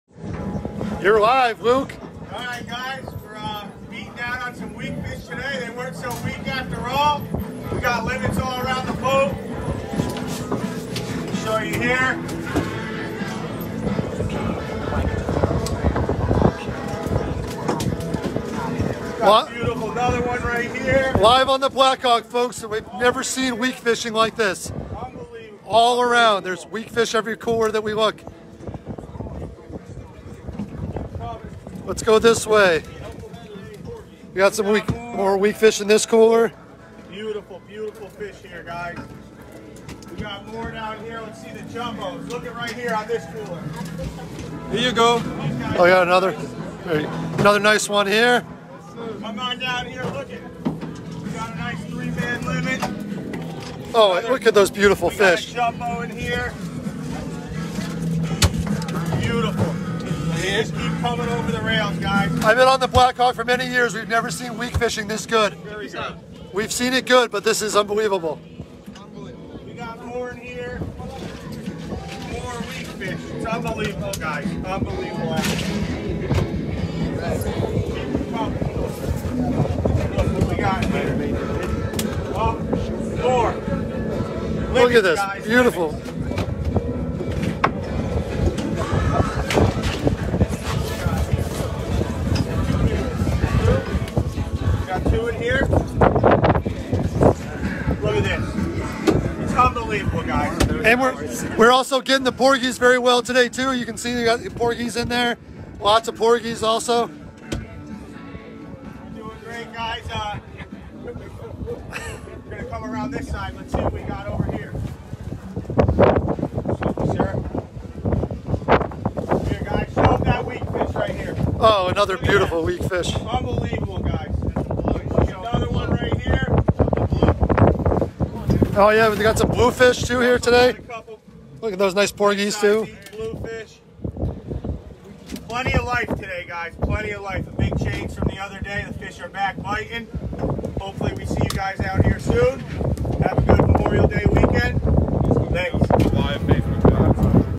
Weakfish on the Black Hawk
Look at the weakfish!  From our live Facebook video on the Black Hawk